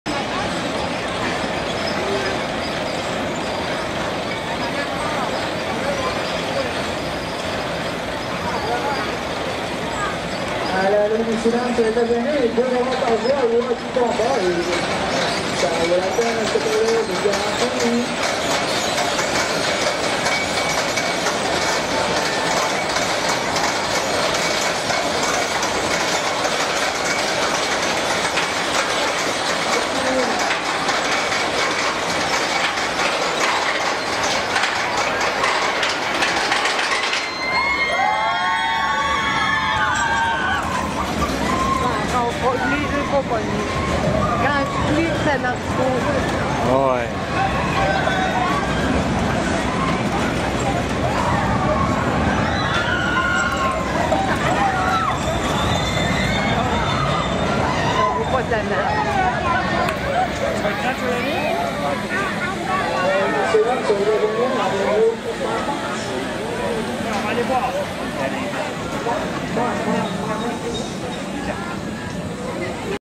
MONTAGNE RUSSE
MONTAGNE RUSSE AU ZOO DE GRANBY (ANACONDA) KALINKA ET ANDREANNE